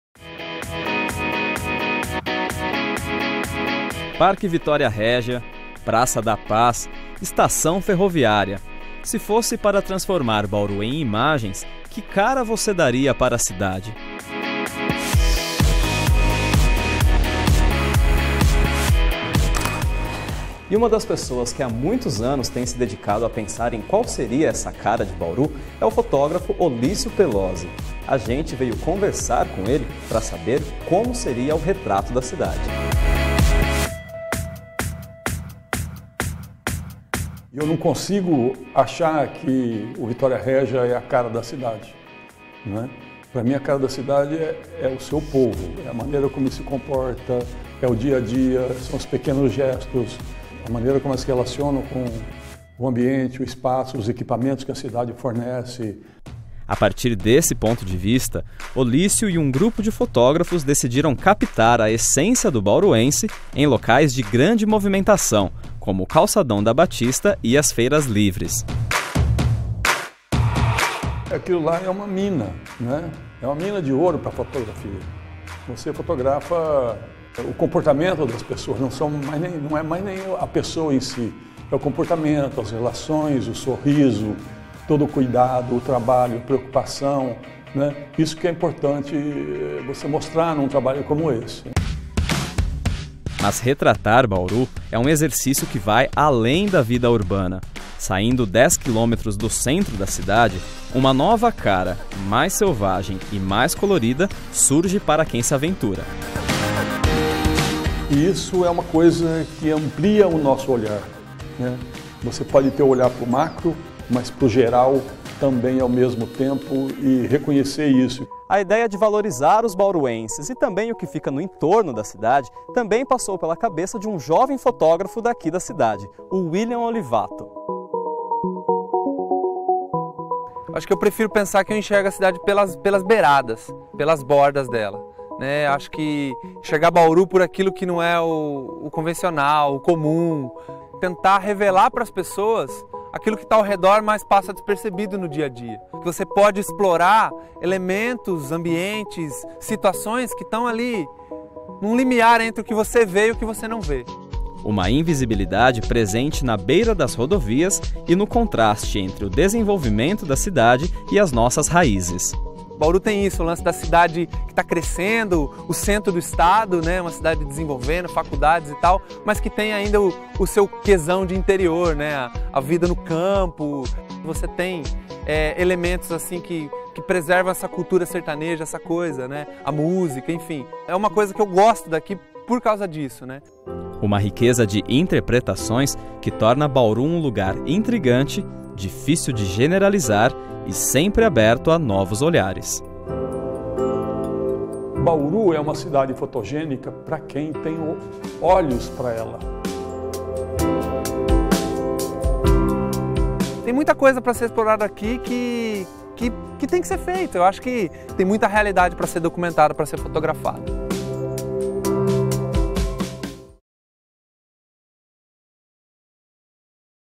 Matéria Unesp Notícias - 02/11/2012 - Cotidiano de Bauru inspira fotógrafos
Dois fotógrafos de Bauru mostram que a beleza da cidade está nos pequenos detalhes, em cenas do cotidiano.